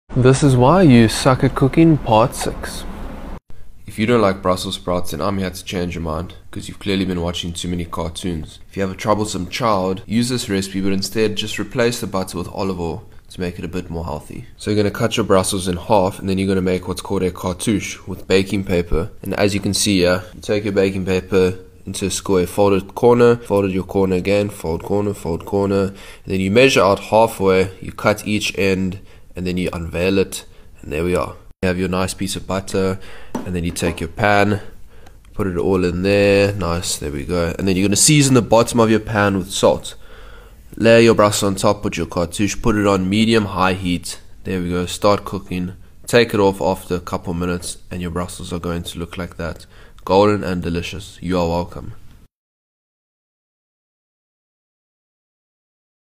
Eat MY BRUSSEL SPROUTS and you'll cry of happiness
I share my method about how I cook brussel sprouts. Every person who eats my brussel sprouts are blown away because they taste so insane! I cook them in some butter and salt and love.